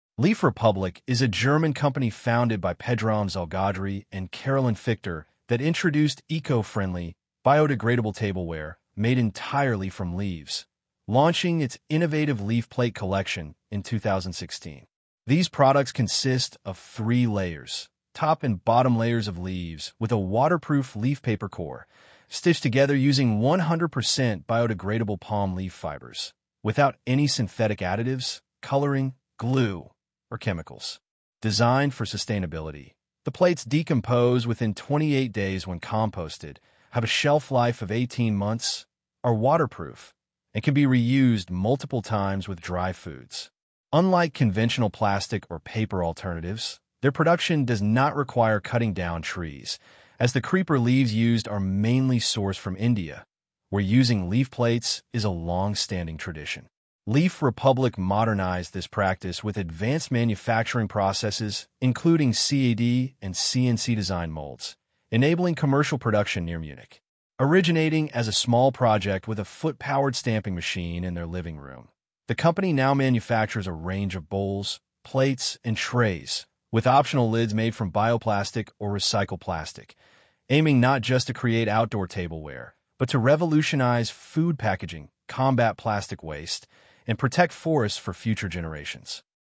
Leaf Republic: Plates Made Entirely from Leaves
Discover how Leaf Republic, a German startup, is transforming food packaging with eco-friendly, biodegradable plates made entirely from leaves. Founded by Pedram Zolgadri and Carolin Fiechter, their innovative design uses no plastic, glue, or chemicals—just nature. These waterproof, compostable plates decompose in 28 days and require no trees to be cut. Learn how ancient Indian traditions inspired a sustainable future in Western markets.